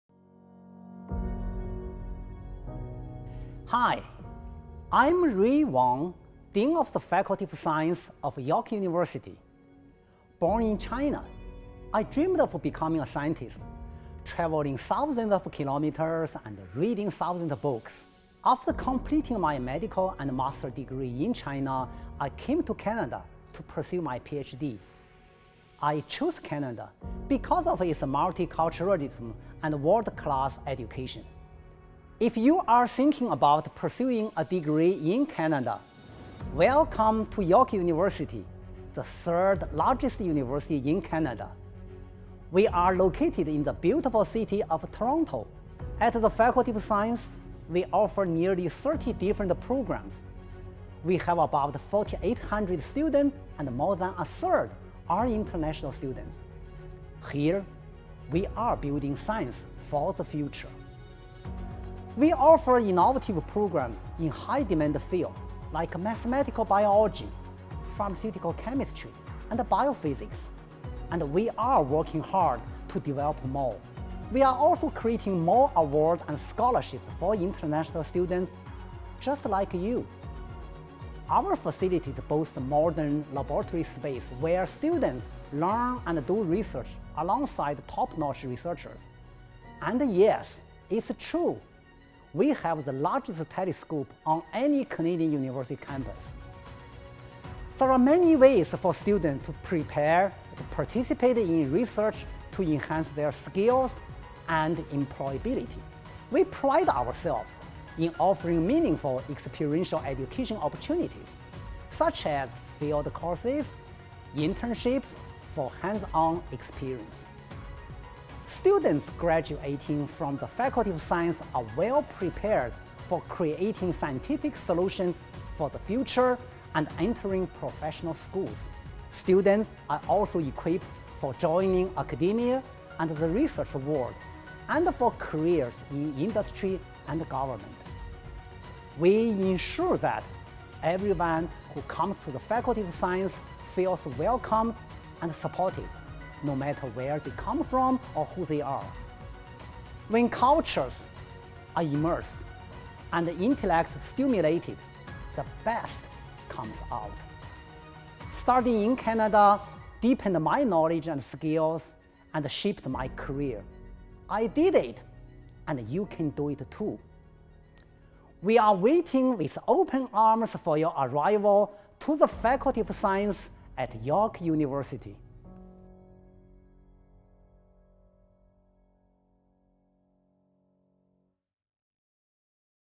Message from Dean Rui Wang to prospective international students
The Dean of the Faculty of Science at York University offers a message of welcome to prospective students living abroad who are thinking of studying in Canada.